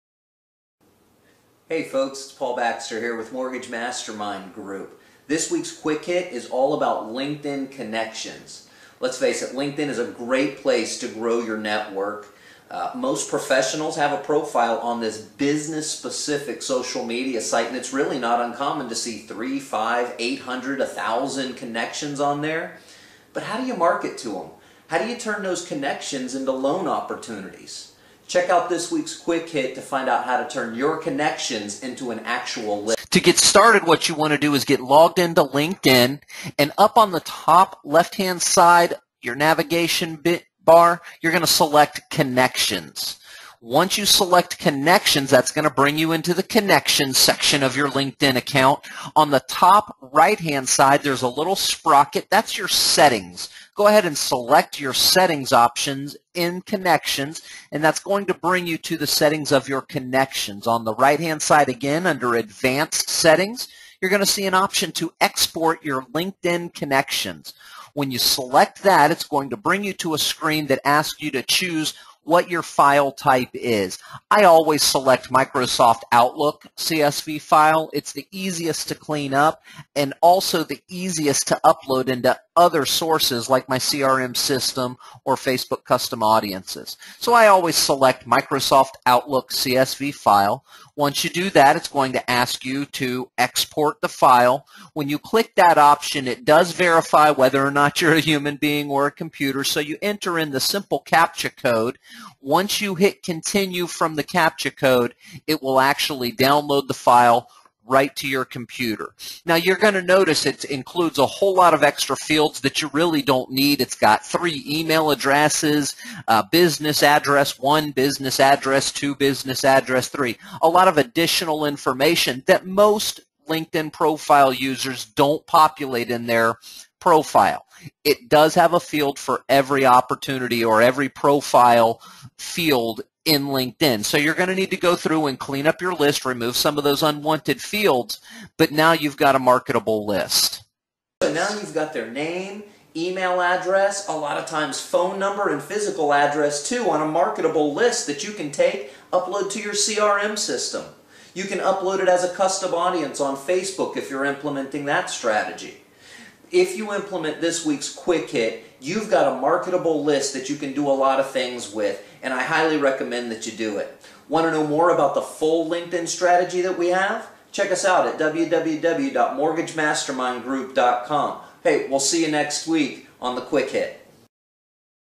Mortgage Lender Marketing Strategies | Paul Baxter
Check out this Mortgage Lender marketing strategy. Paul Baxter, Implementation Coach for Mortgage Mastermind Group shows a cool marketing strategy for mortgage lenders. This mortgage lender marketing strategy is about how to export your Linked In connections.